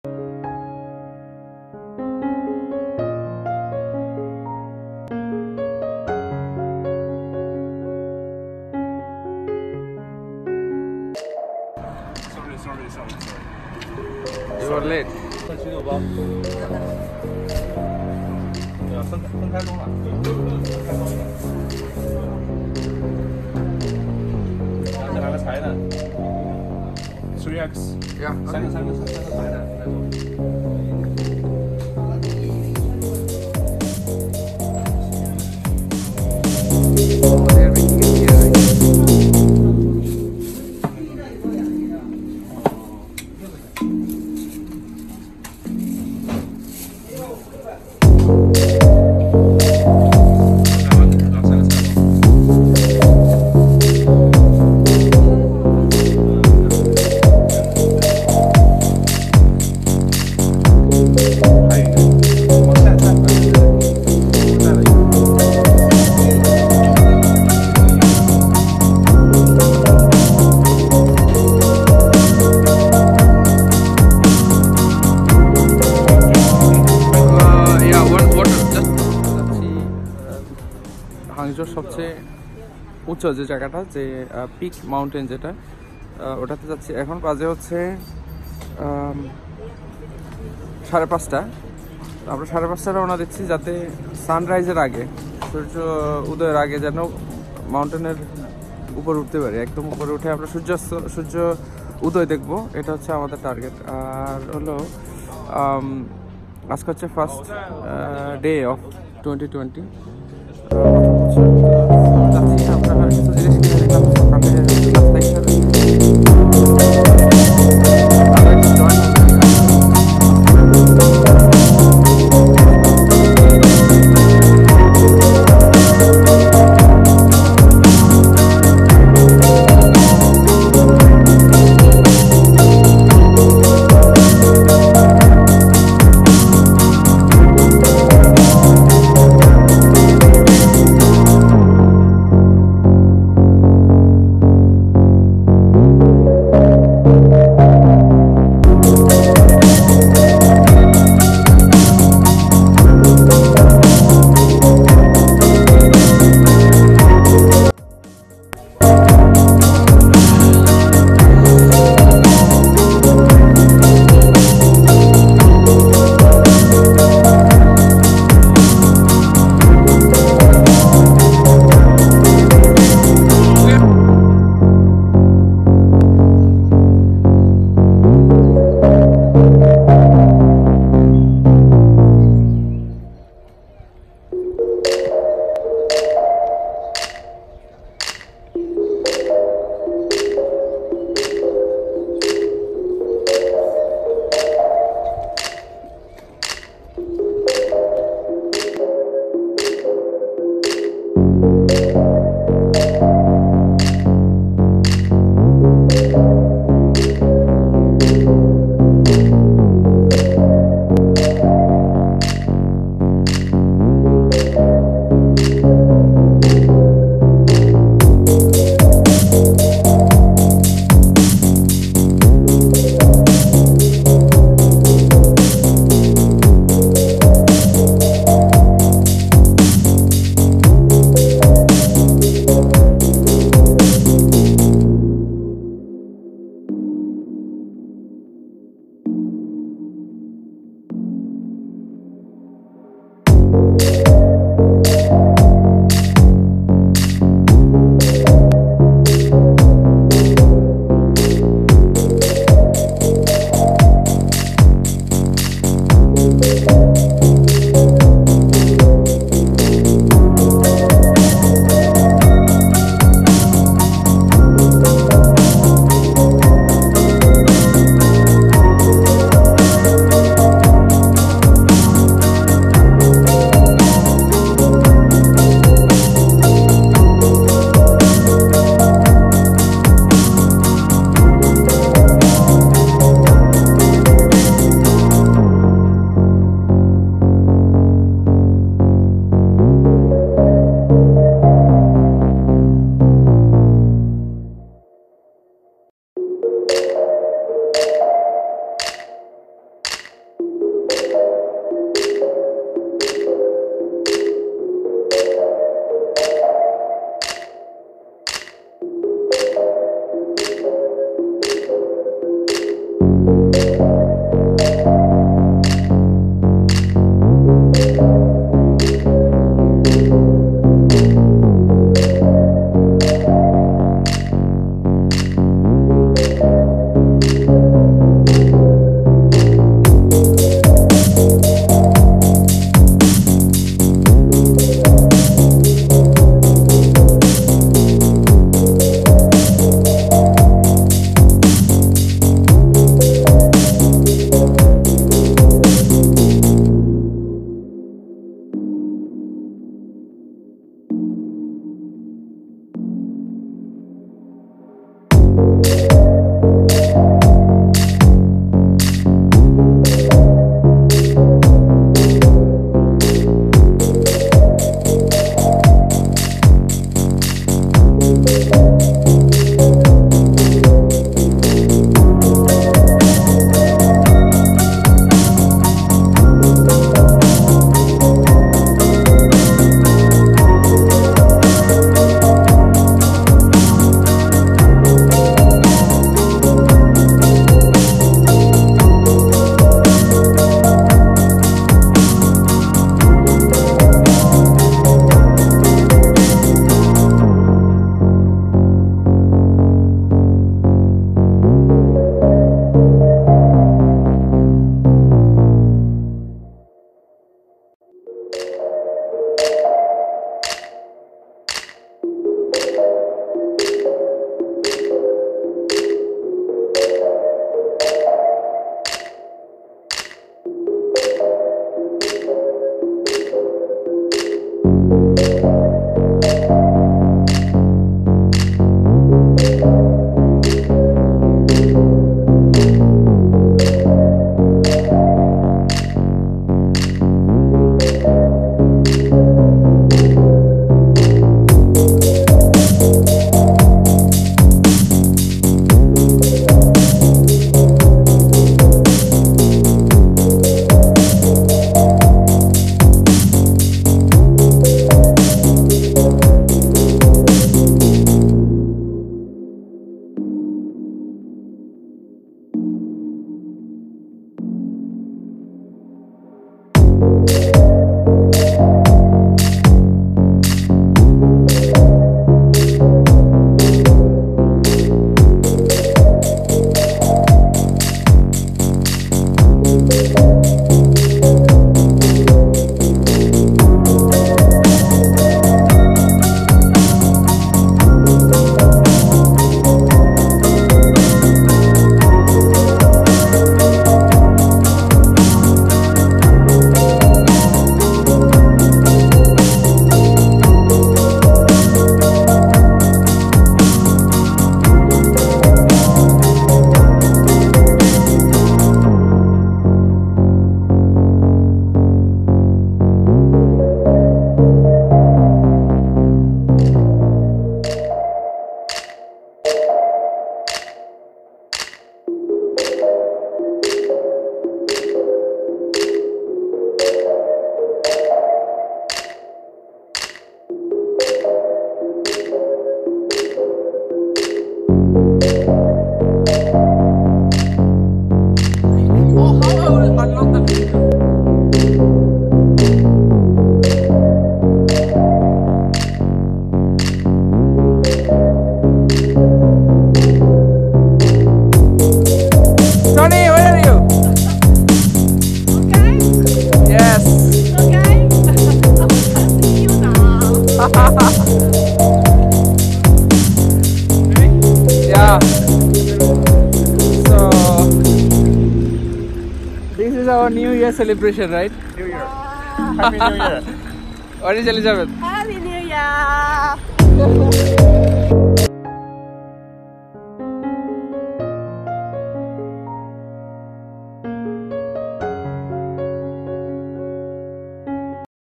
First Sunrise of 2020 in Hangzhou
We tried to welcome the first sunrise of 2020 from the highest peak of Hangzhou, "The Northern Peak". A beautiful place indeed.